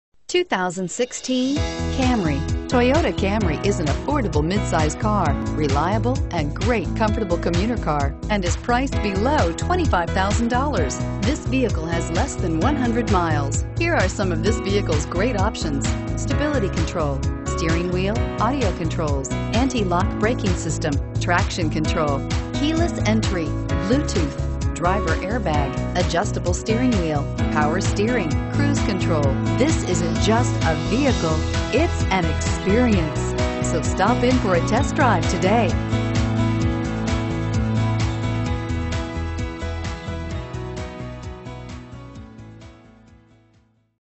2016 Toyota Camry 221360
2016 TOYOTA CAMRY Richmond, VA
Stock# 221360

McGeorge Toyota
9319 West Broad Street

Looking for the right car? Today could be your lucky day. With 8 miles, this Gray, 2016 Toyota Camry equipped with Automatic transmission could be yours. Request more information and set up a test drive right away.